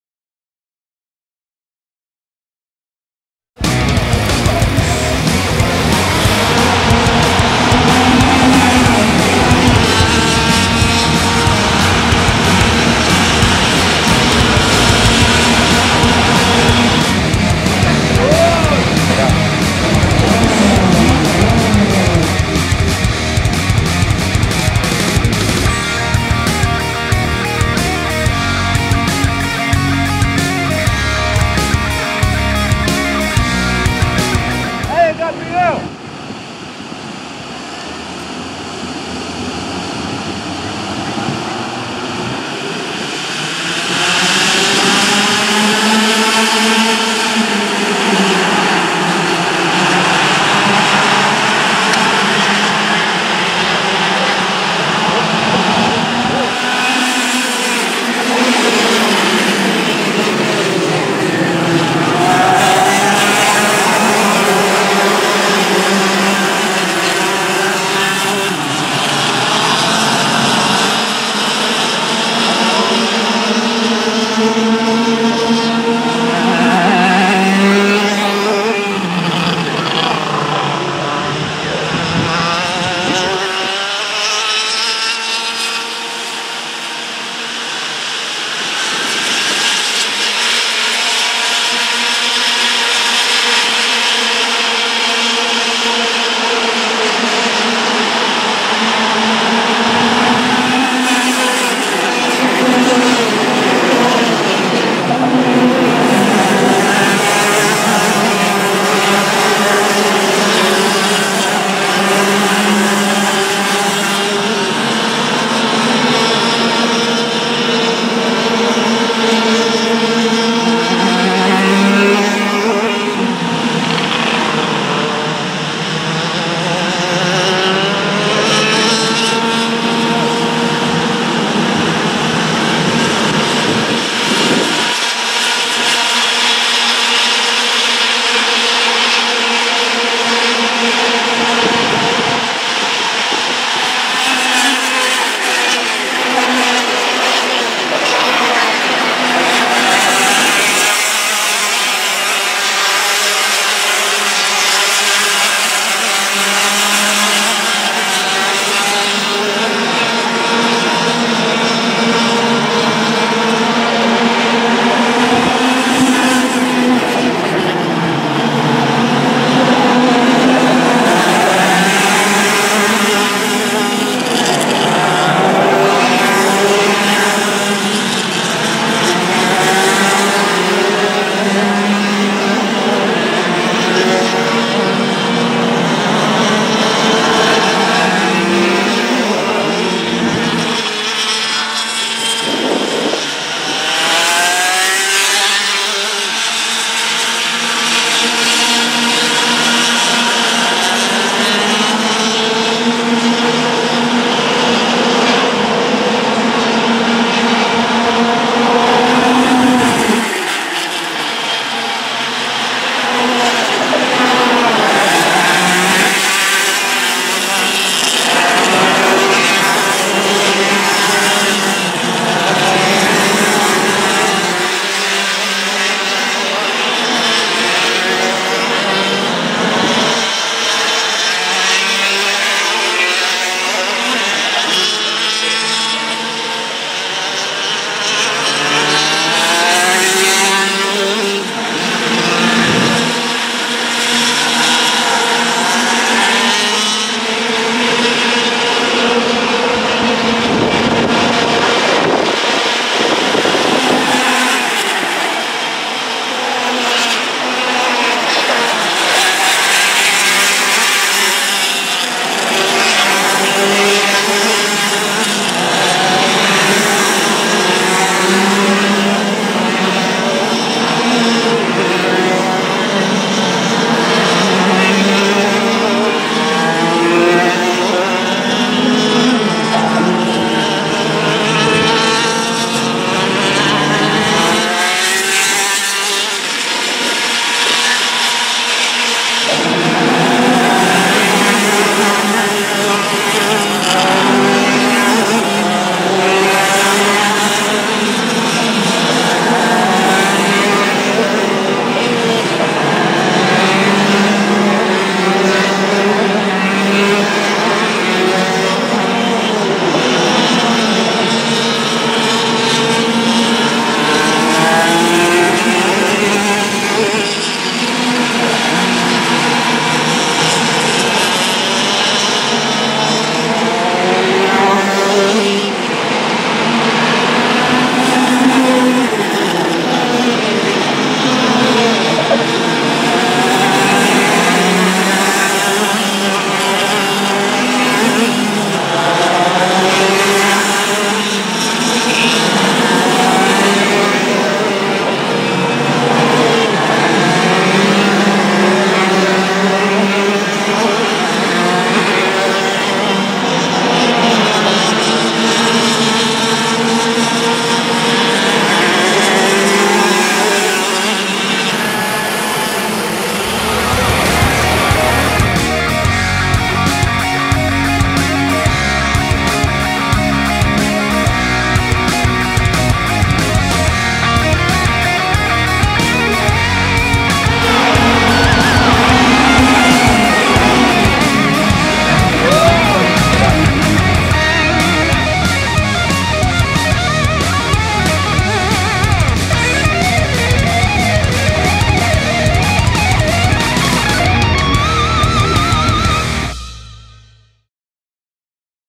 3ª Et. Copa São Paulo Light de Kart 2017 - #80 - Fabricio Fardim / Junior Menor 1ª prova
Video on-board do piloto Fabricio Fardim  na primeira prova da categoria Junior Menor durante a 3ª Etapa da Copa São Paulo Light de Kart que foi realizado no dia 01 de Abril no Kartódromo Aldeia da Serra - São Paulo/Brasil.